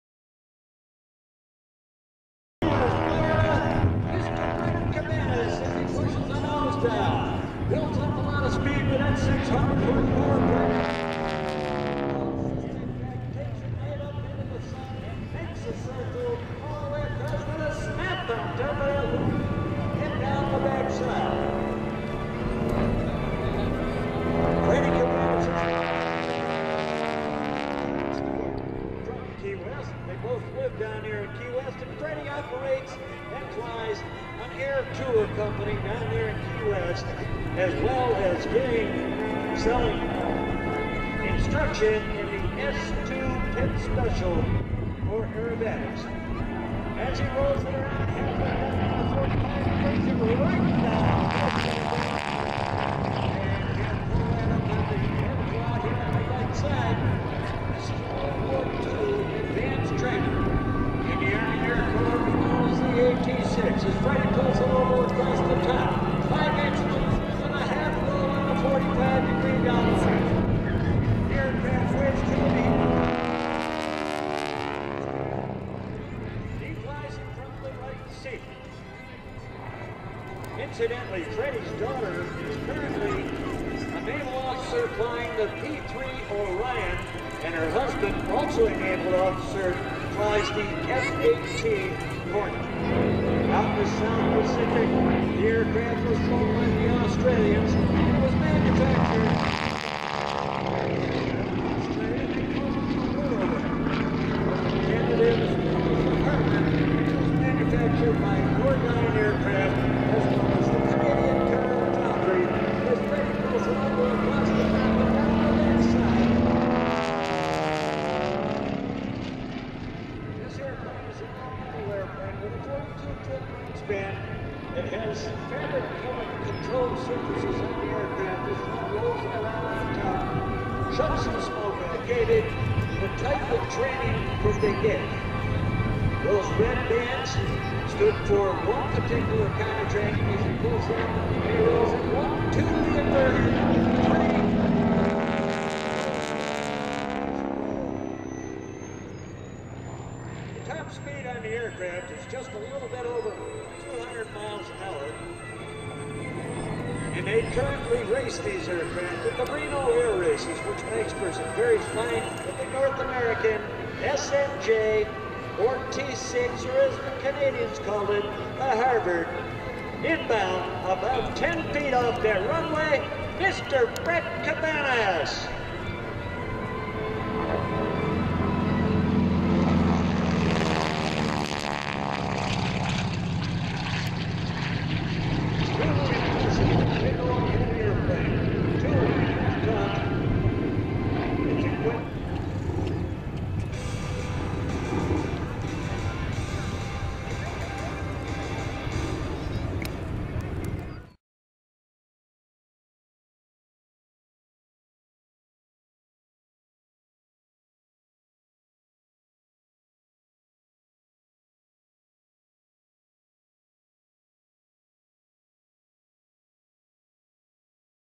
R.I.P. Fred Cabanas T-6 Texan @ Cecil Field AirShow 2009
Fred Cabanas started flying at the age of 16 and has over 20,000 hours of total flight time. He holds an Airline Transport Pilot, Multi-Engine, Instrument, Commercial, and Seaplane rating. Fred is also rated in the North American T-28 and Hawker Sea Fury. At the Reno air races in September 2005 Fred won the Silver on Saturday and was advanced to the Gold on Sunday to take 8th place overall making Reno history and was selected Rookie of the year. He performs air shows internationally in the PITTS SPECIAL S-2C and Hawker Sea Fury, N. American T-28, N. American T-6, N. American P-51 Mustang, Waco, and J3 cub. Fred is a member of the Screen Actors Guild. He has been featured in numerous movies and TV Programs including MTVs Road Rules.